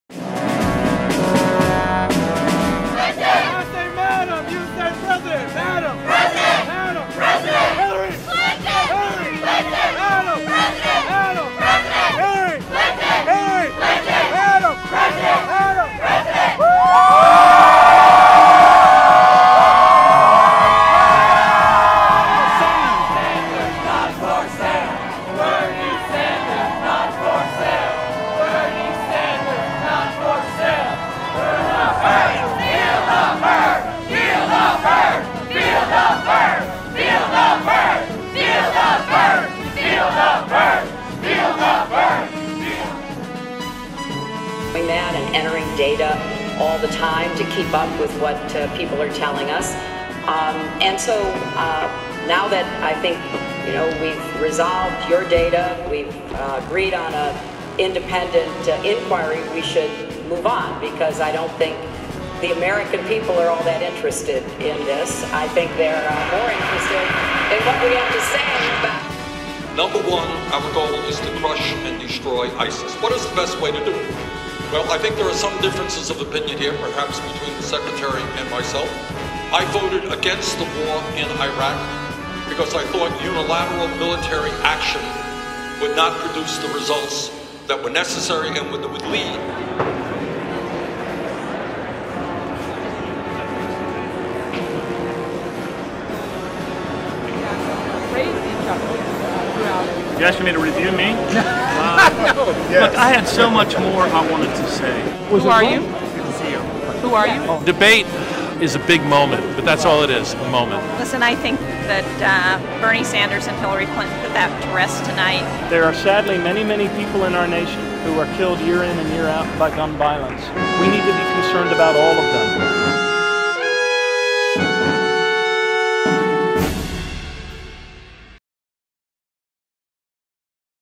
New Hampshire Debate Coverage You Didn't See
Here is a different look at the happenings around the Dec. 2015 Democratic debate with Hillary Clinton and Bernie Sanders.  Not so much "behind the scenes" but a view that the media does not show people.